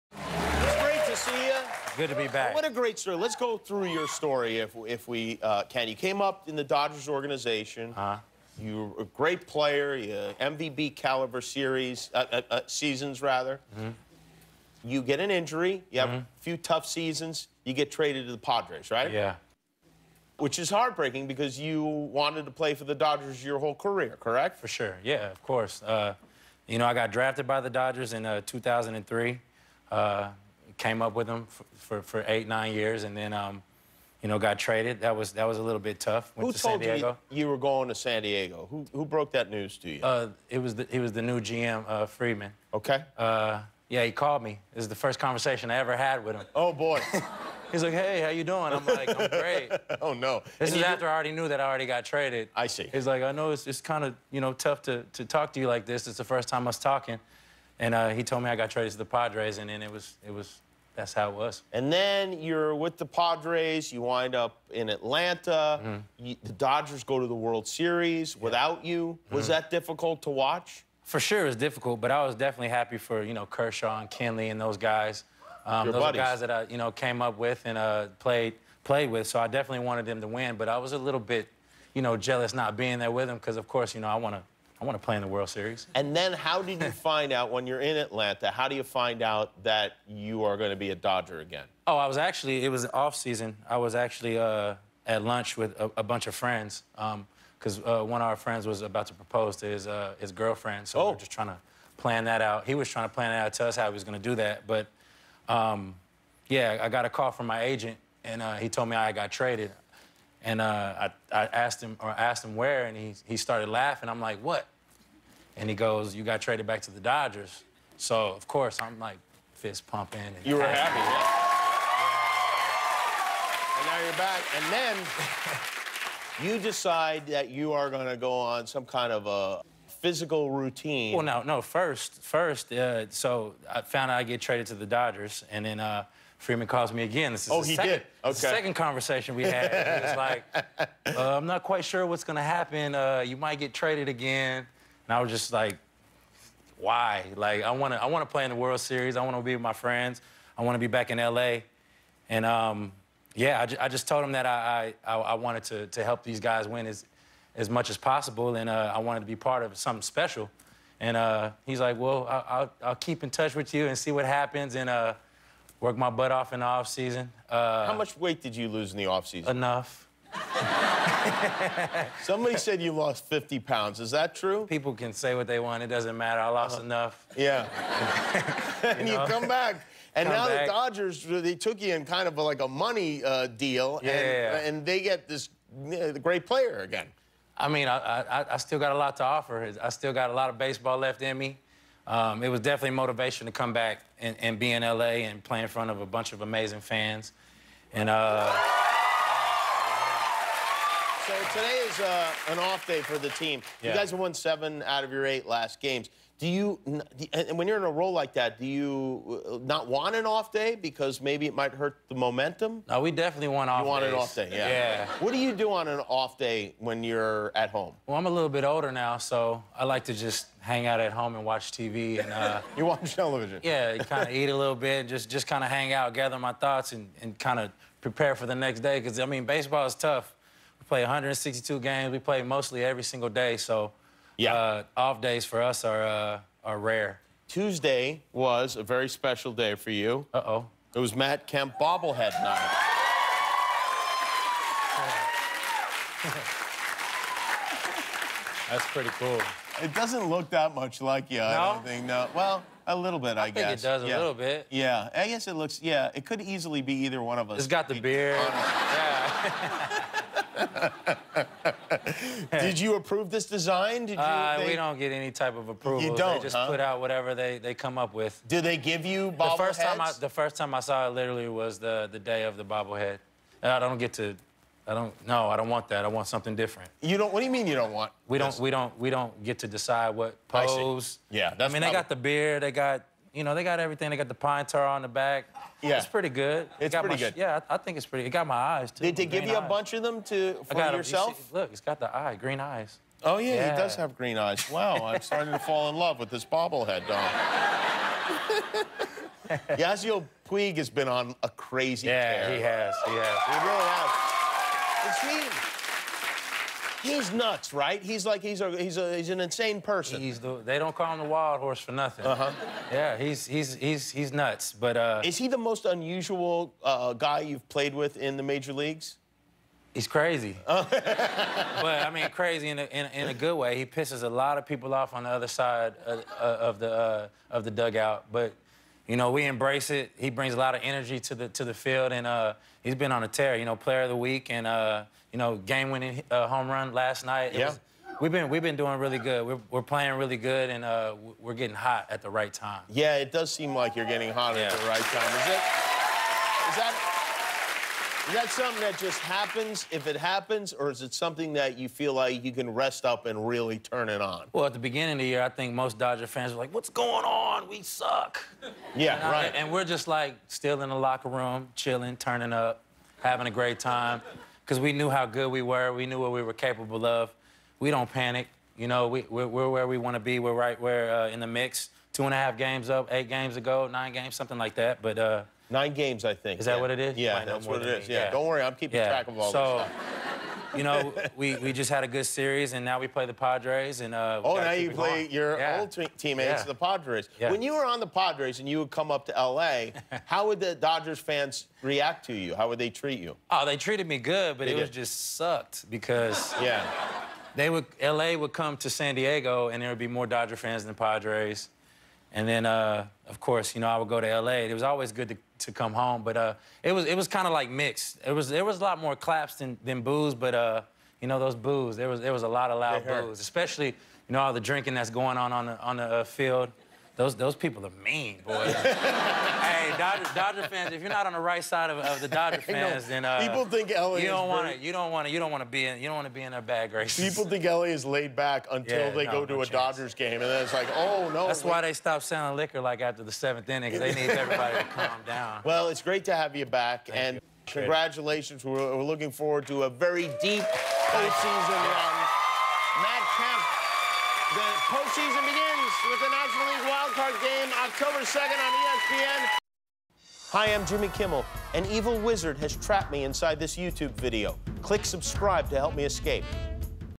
Matt Kemp on Returning to the LA Dodgers
Matt talks about being traded and coming back to the LA Dodgers, his new bobblehead and the team's recent hot streak.